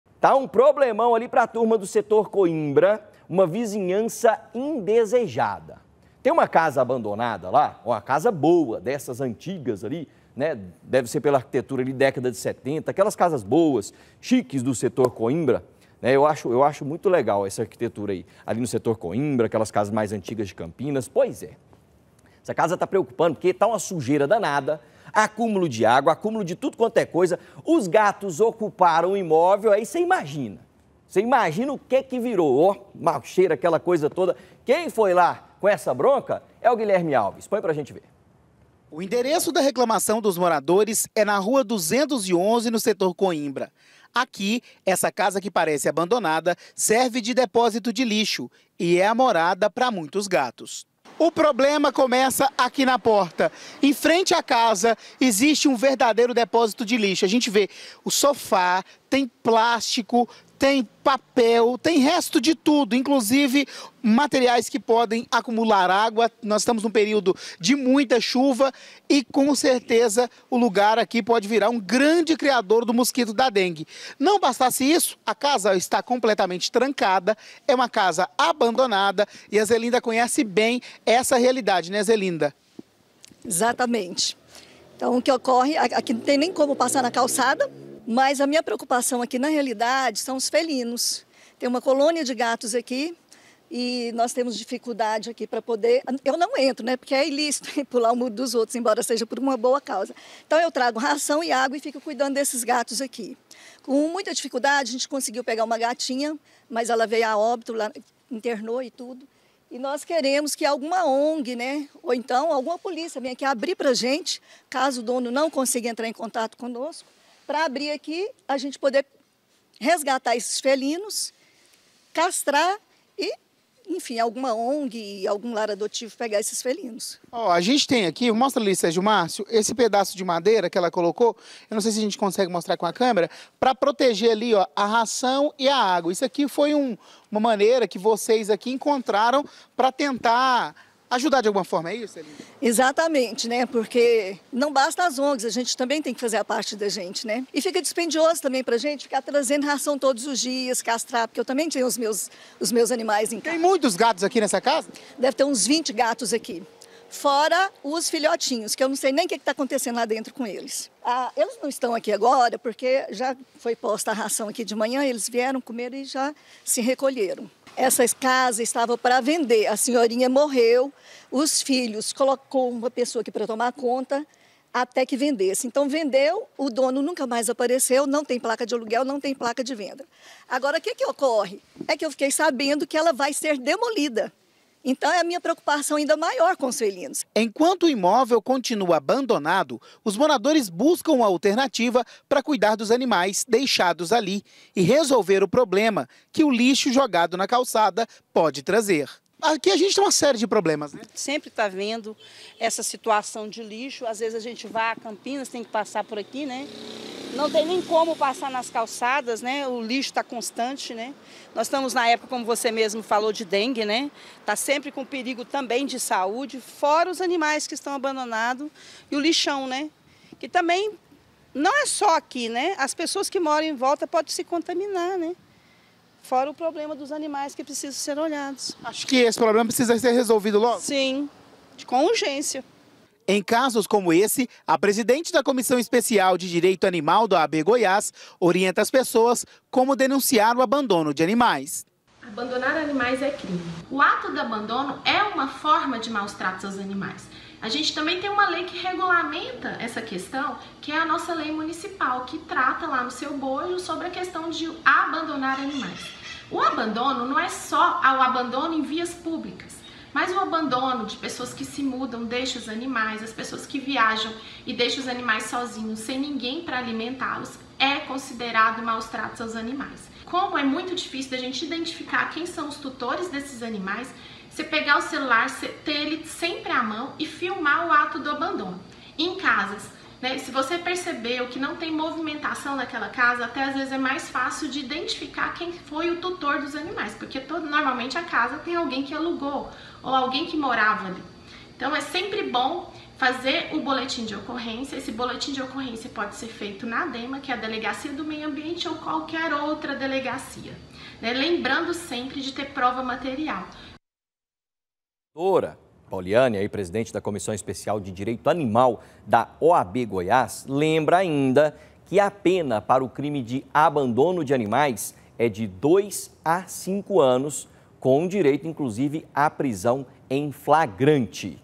VIZINHANÇA INDESEJADA: SUJEIRA E MUITOS GATOS EM MOCÓ
UMA CASA ABANDONADA NO SETOR COIMBRA, EM GOIÂNIA ESTÁ PREOCUPANDO A VIZINHANÇA. ALÉM DE MUITA SUJEIRA ACUMULADA, VÁRIOS GATOS OCUPAM O IMÓVEL.